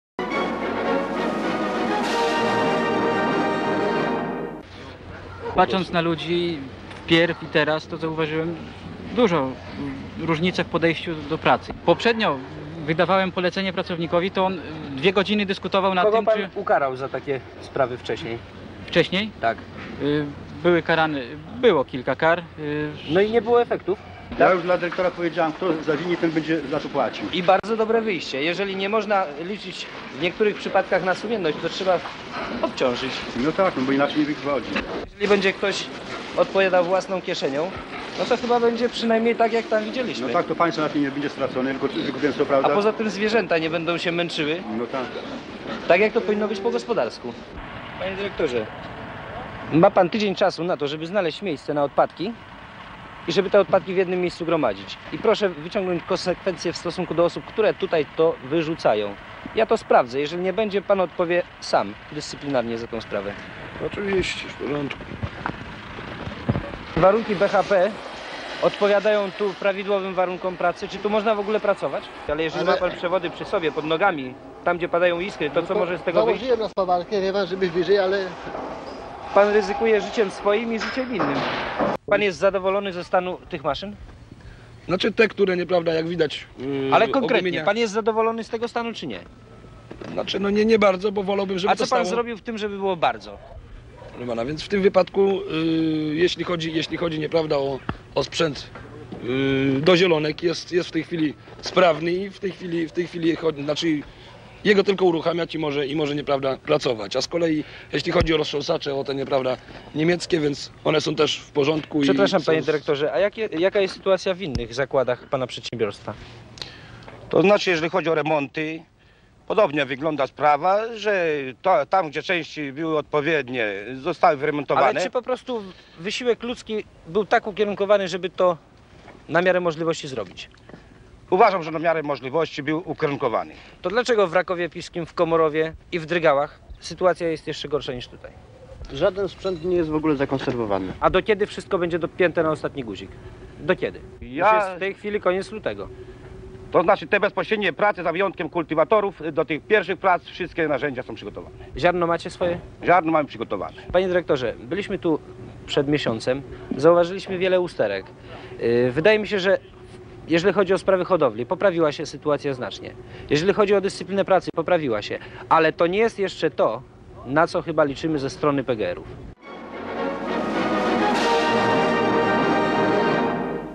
PRL 1982 Stan wojenny Komisarz wojskowy w PGR
PRL 1982 Stan wojenny. Komisarz wojskowy kontroluje stan ładu i porządku w PGR w województwie suwalskim. Zapraszam na Facebook TVPolandAntena